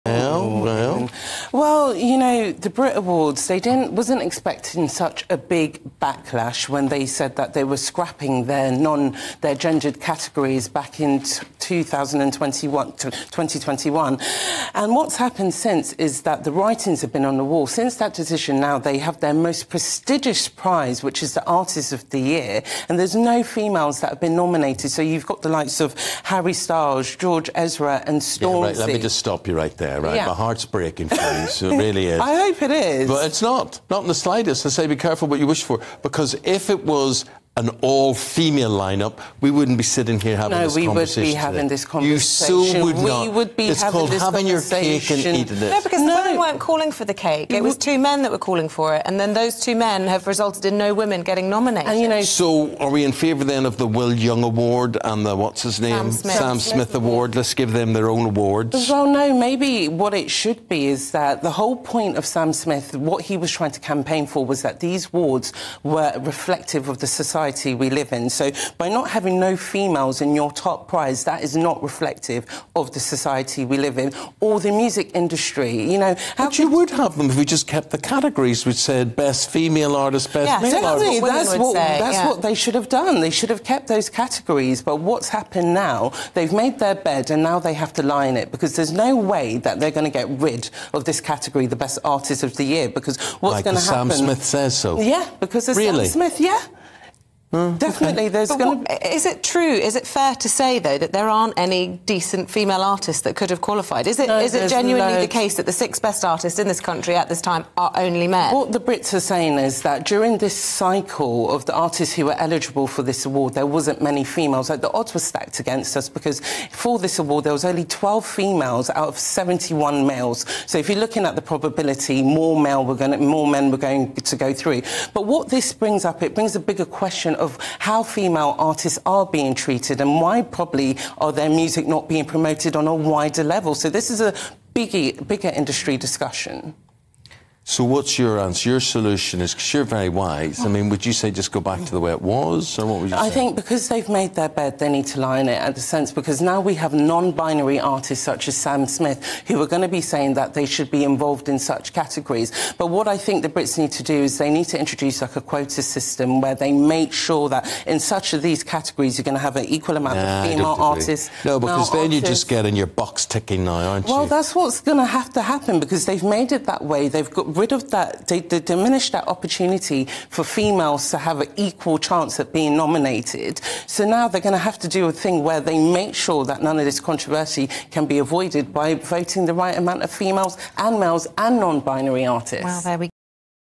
The Brit Awards SLAMMED over gender-neutral category | 'They've made their bed they can lie in it'
'They've made their bed and now they have to lie in it... there's no way they're going to get rid of this category.'

Showbiz journalist @stef_scoop discusses The Brit Awards being slammed for introducing a gender neutral category this year but only nominating men.